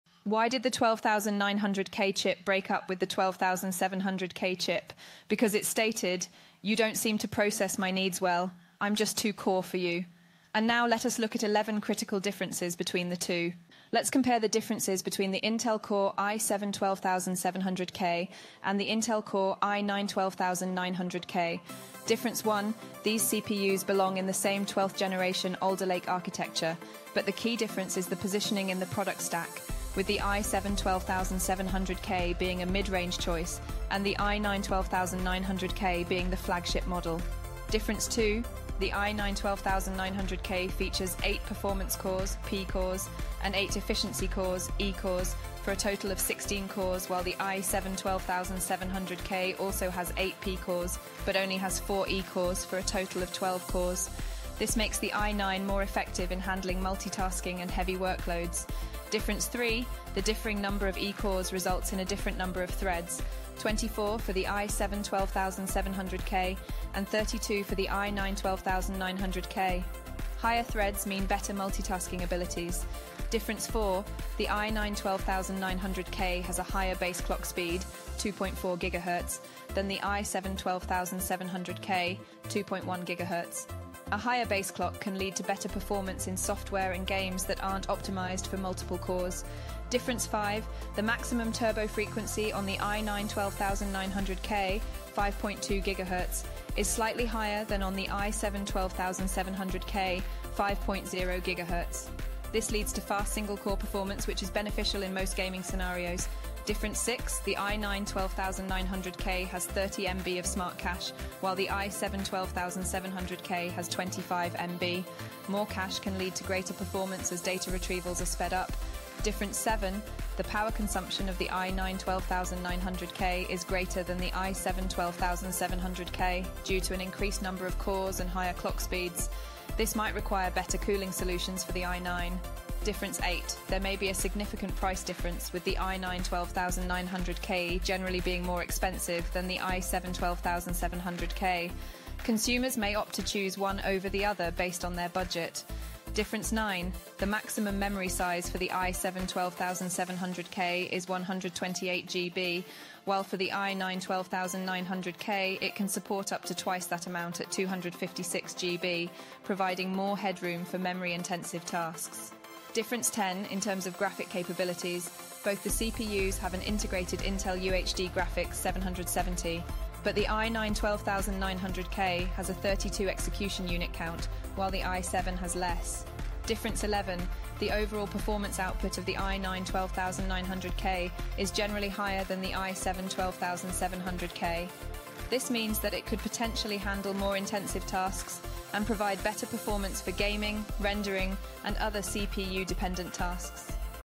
Comparing the Intel Core i7-12700K vs i9-12900K: 11 Key Differences
In this informational video, we delve into the defining differences between the Intel Core i7 12700k and the Intel Core i9 12900k. Both belong to Intel's 12th generation Alder Lake architecture, but which one truly outperforms the other? We compare their positioning in the product stack, core counts, thread counts, base clock speeds, maximum turbo frequencies, Smart Cache sizes, power consumptions, prices, maximum memory sizes, graphic capabilities and overall performance outputs. Uncover which CPU is the better choice for your specific needs, whether for gaming, rendering, or other CPU-dependent tasks. Don't miss out on these key insights - click to watch now!